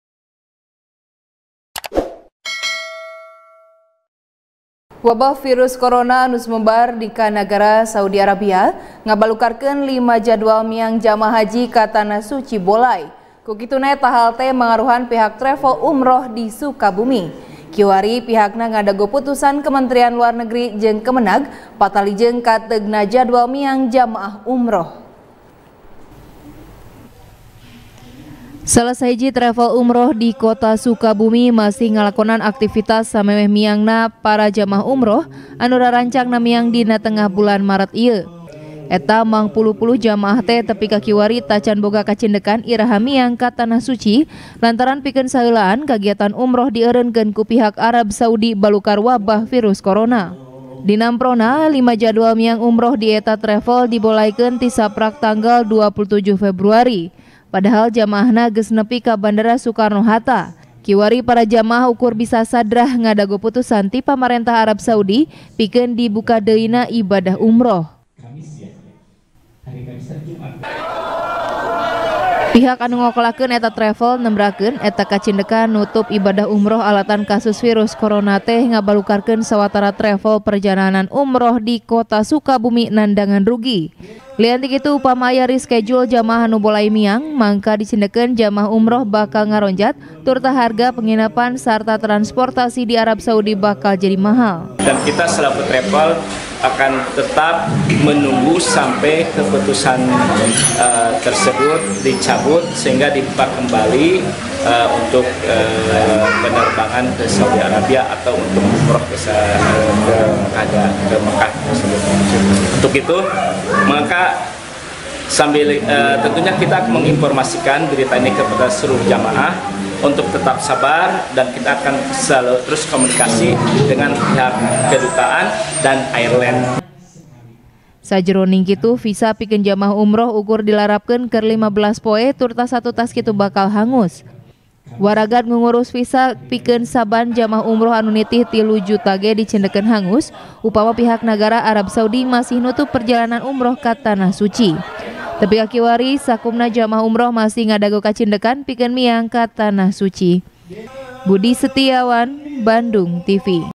Jamaah Umroh Bolay Miang Ka Tanah Suci | TP | BANDUNGTV
TP | BANDUNGTV | ELBY NOPITRI | Wabah Virus Corona Anu Sumebar Ka Nagara Saudi Arabia, Ngabalukarkeun Lima Jadwal Miang Jamaah Haji Ka Tanah Suci Bolay. Ku Kituna, Eta Hal Teh Mangaruhan Pihak Travel Umrah Di Sukabumi. Kiwari, Pihakna Ngadago Putusan Kementrian Luar Negeri, Jeung Kemenag, Patali Jeung Kateugna Jadwal Miang Jamaah Umroh.